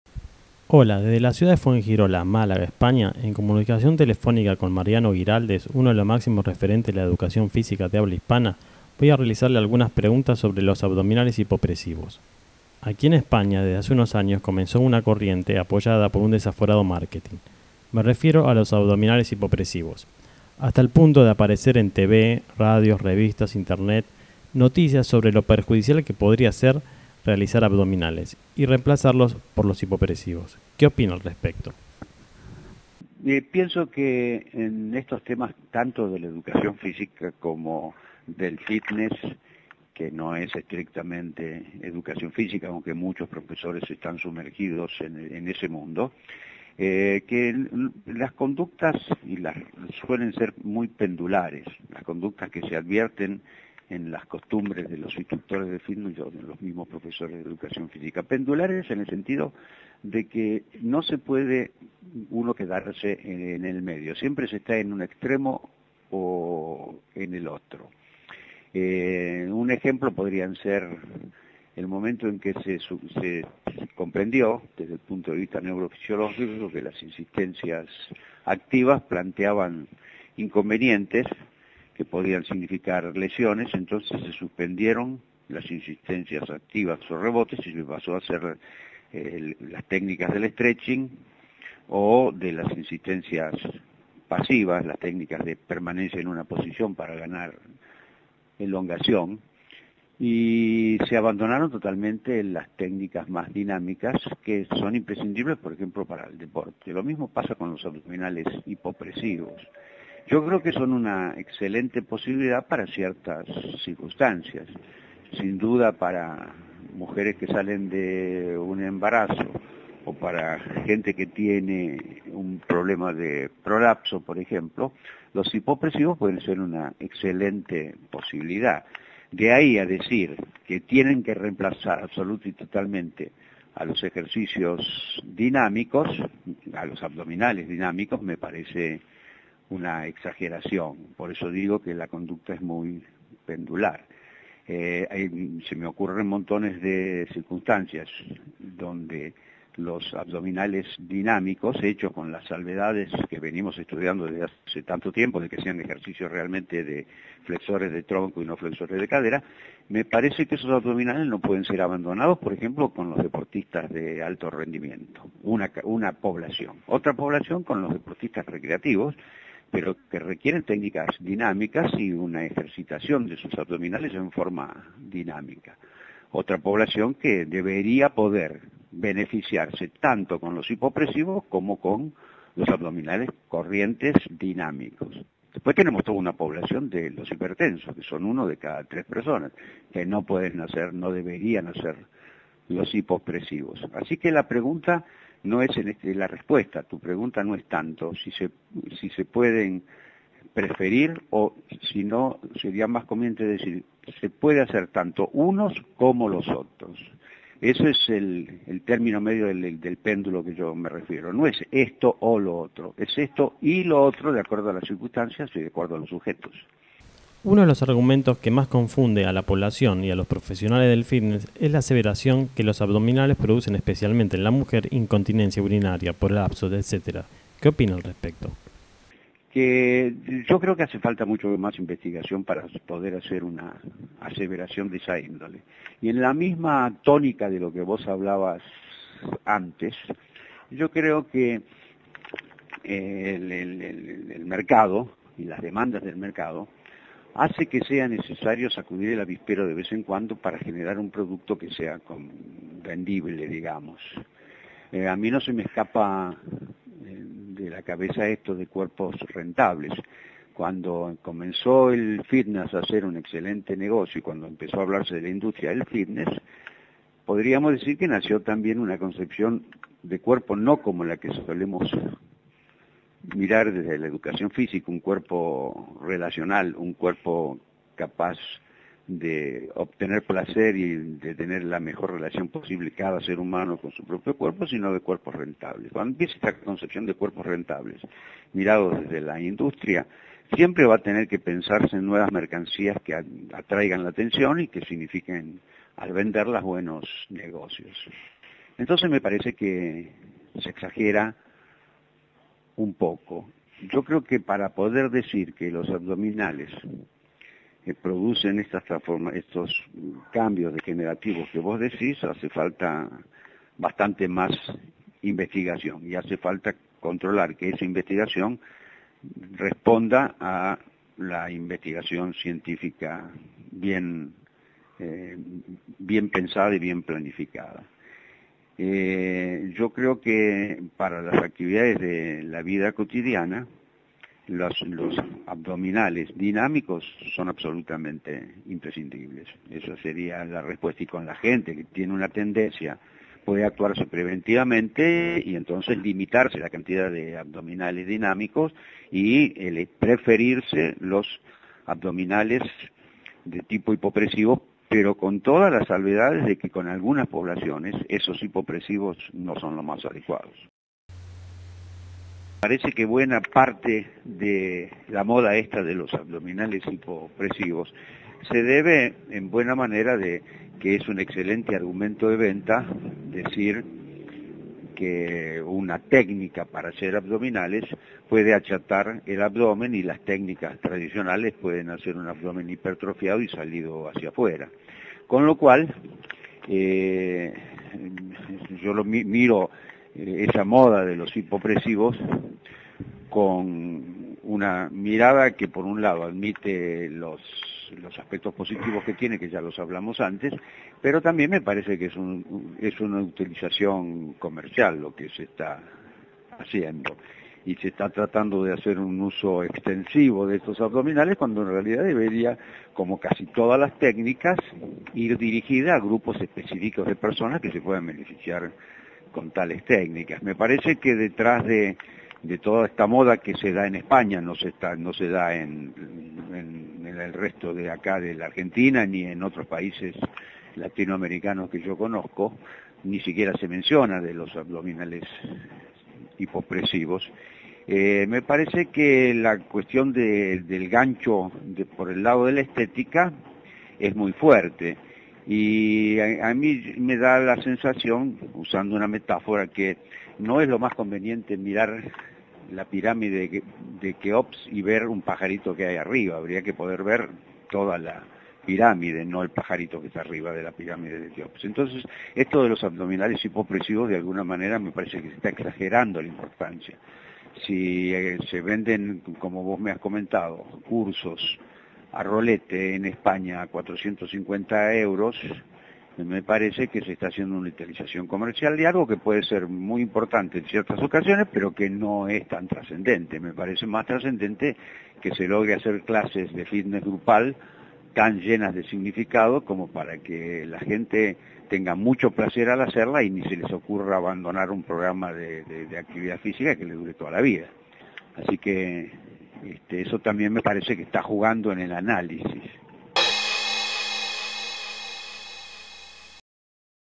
Abdominales Hipopresivos: ¿Negocio, realidad, mito?
En este video Mariano entrevista al Profesor Mariano Giraldes, uno de los máximos referentes de la Educación Física de habla hispana. Mariano Giraldes explica los beneficios de está técnica, afirma que los ejercicios abdominales que involucran la flexión del tronco no deberían dejar de realizarse y nos ofrece su visión sobre el negocio que rodea a la industria del fitness para vendernos diversos productos en forma constate.